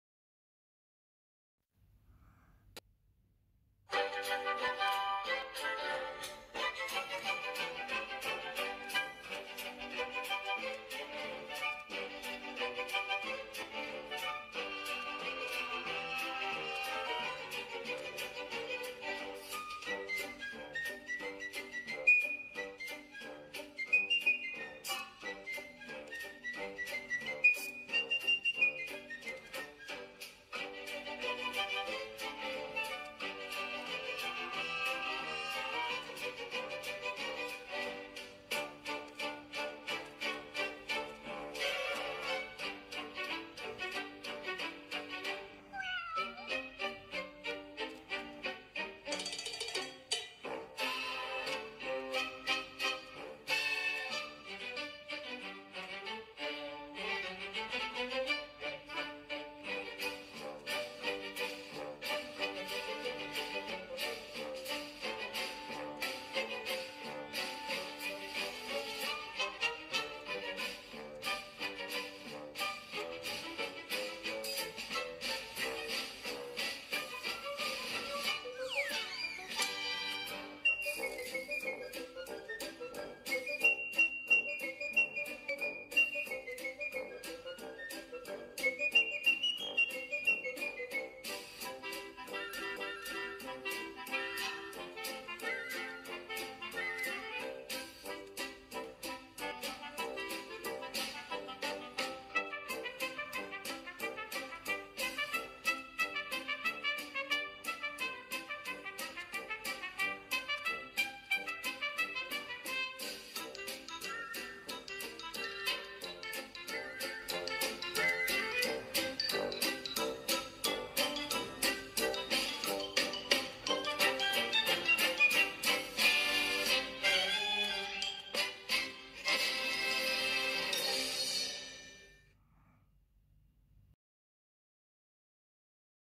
Minnie mouse magneteic photo frame (DIY)برواز للثلاجة من الخرز الحراري
Enjoy sharing with me the preparation of my winner photo-frame perler beads.

✨ MY TOOLS:

►Fuse beads خرز حراري
►Paper ورق زبدة
►Magnet Sheet فرخ مغناطيس